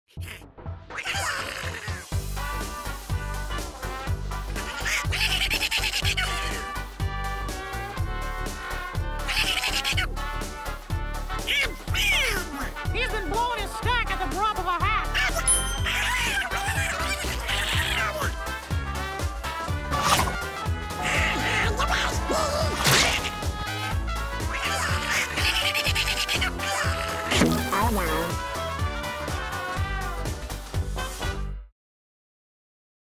Proyección de cortos 85º Aniversario del Pato Donald
Disney organiza una selección única de cortos con motivo del 85º aniversario del Pato Donald que incluye algunos de los cortos más icónicos del personaje. Del 15 de junio a las 11:30 h y a las 17:30 h y 16 de junio a las 11:30 h en Espacio Fundación Telefónica.